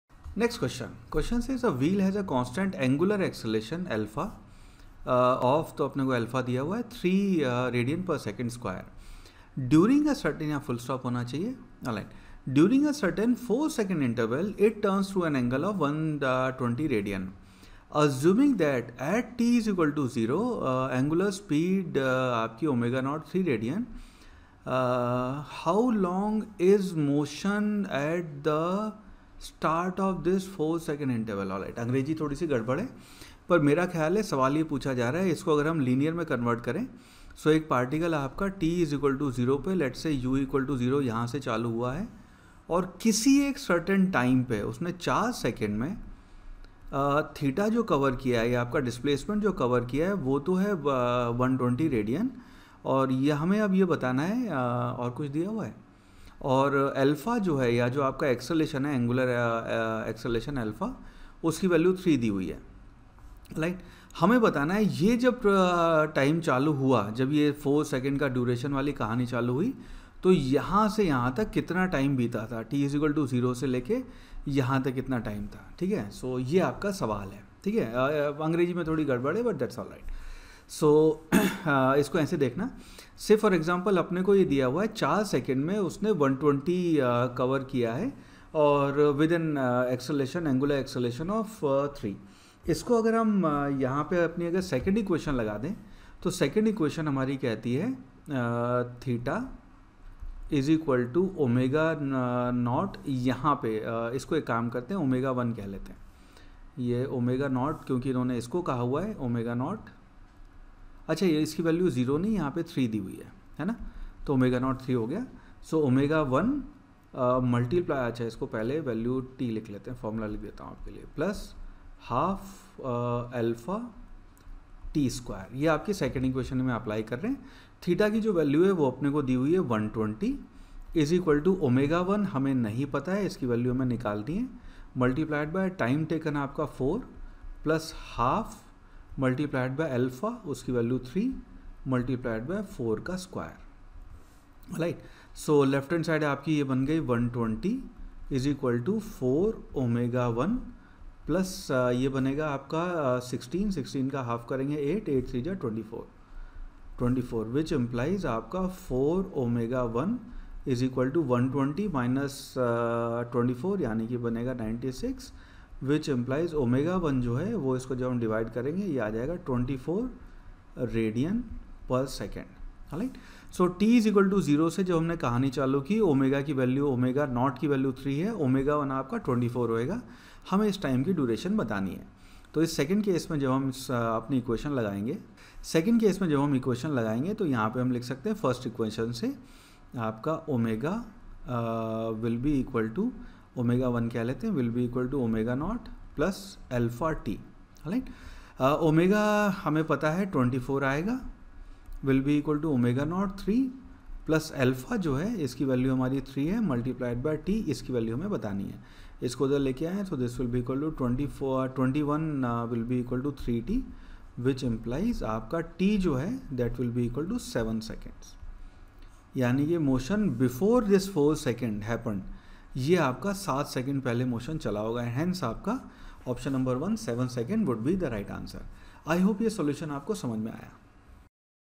A wheel has a constant angular acceleration of 3.0 rad/s2, During a certain 4.0 s interval, it turns
A wheel has a constant angular acceleration of 3.0 rad/s 2, During a certain 4.0 s interval, it turns through an angle of 120 rad. Assuming that at t = 0, angular speed w 0 = 3 rad/s how long, is motion at the start of this 4.0 second interval ?